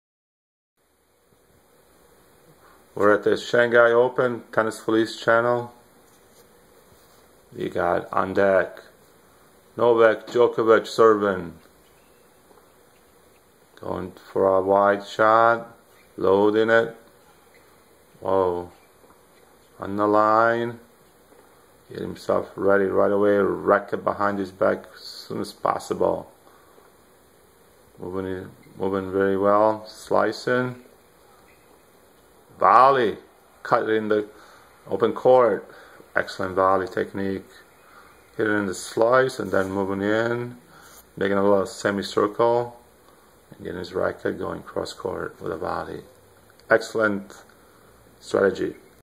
ATP HUGE POINT BY Novak Djokovic vs Feliciano Lopez SHANGHAI Masters 1000
ATP LIVE Novak Djokovic vs Feliciano Lopez SHANGHAI Masters 1000